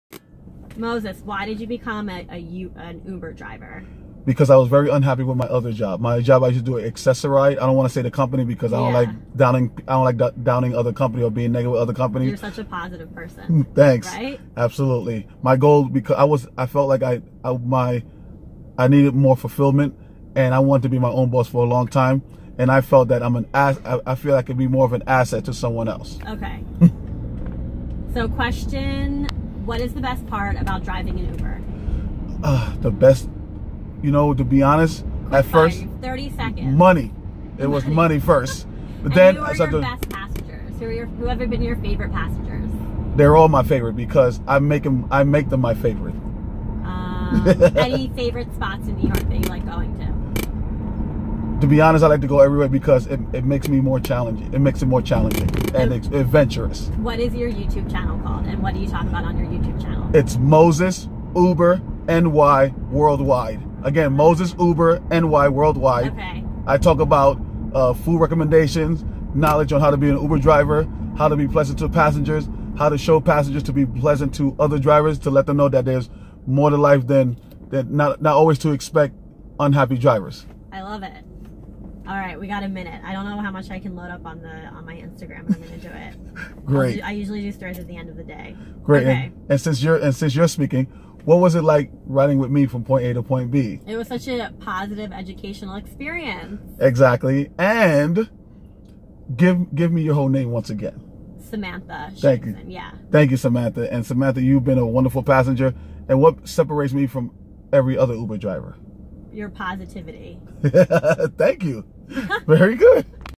Video 126 Samantha The New York Blonde
She is Great did an interview with me as well me with her Great Conversation she into Instagram and Blogging . Her info is Below She's a:
City Blogger
Style Director
NYC Explorer. Ever wanted to manifest more wealth, but tired of all of the fairy tale methods talked about in the law of attraction?